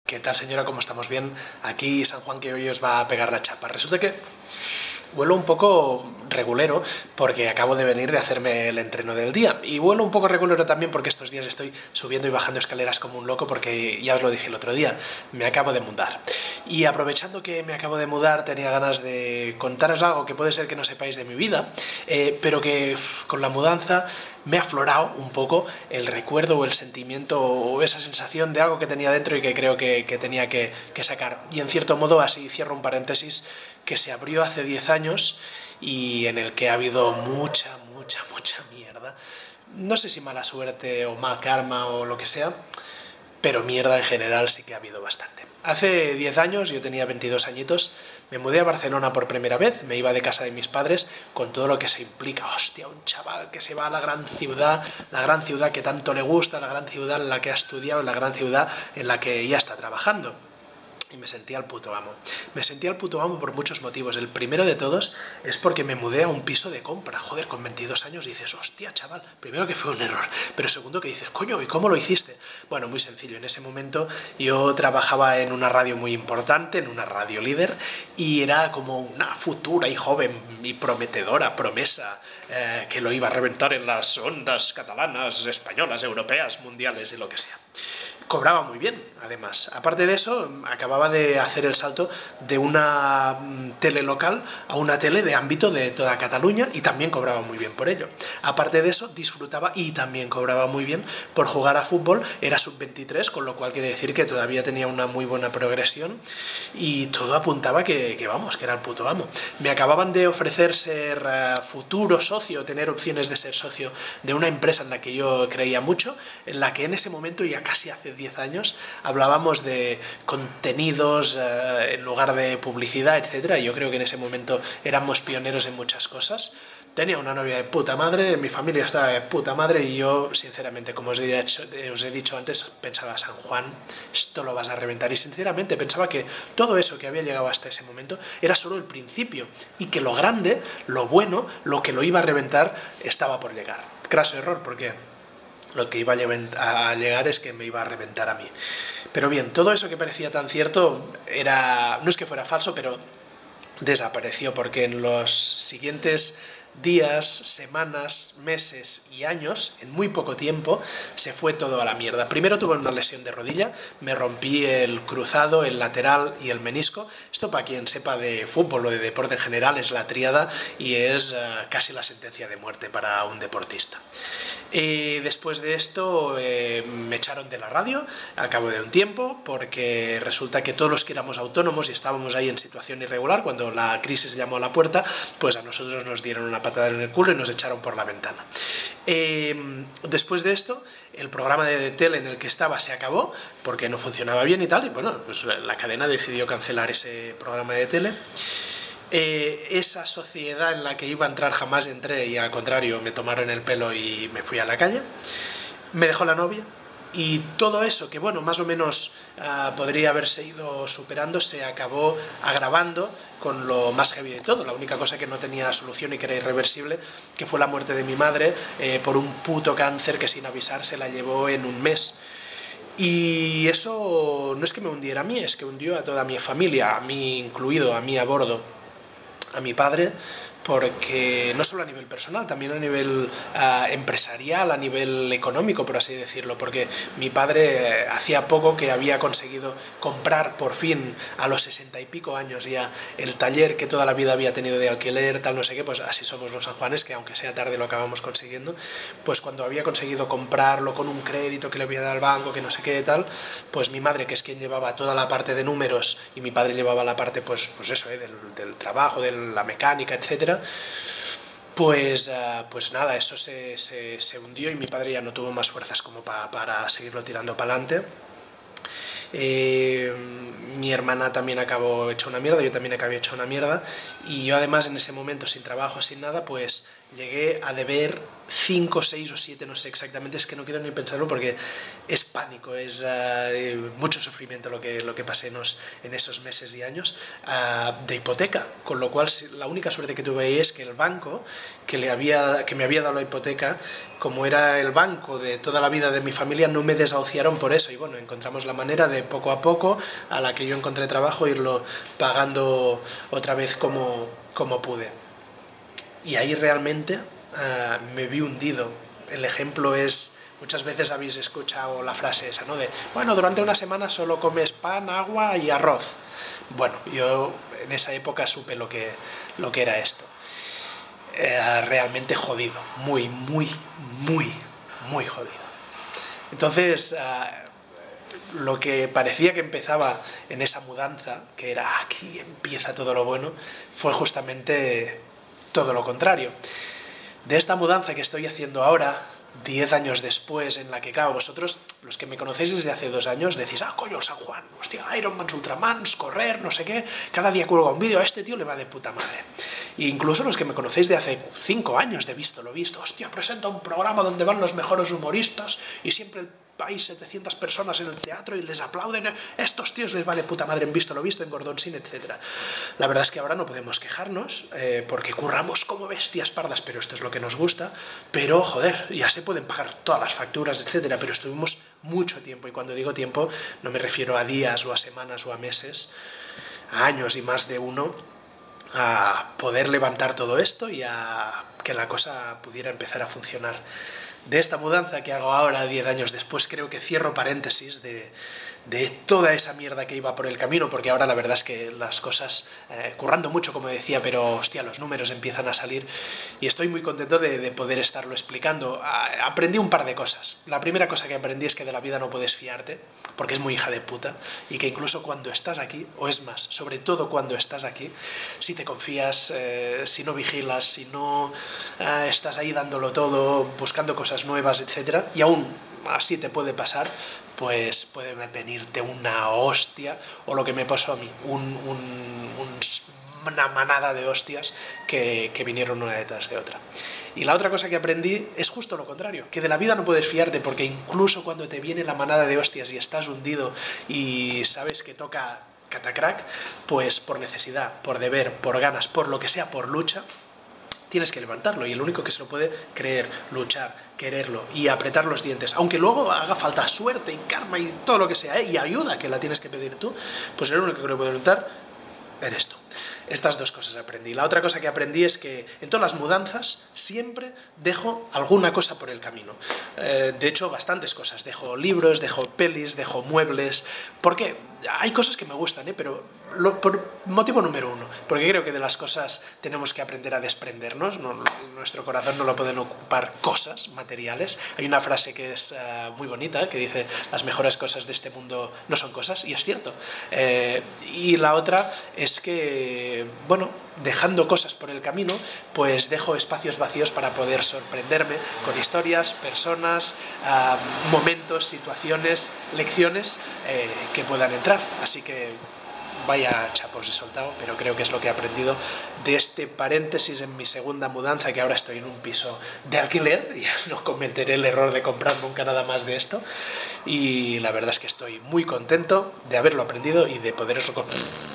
Mudarse De Vida
Cosas que dejaste olvidadas (perdidas?) en tu vida, en una mudanza de un paréntesis de 10 años. Y que, mudándote de nuevo, recuperas. Y cierras el puto paréntesis. Puede que no para siempre, pero sí de momento.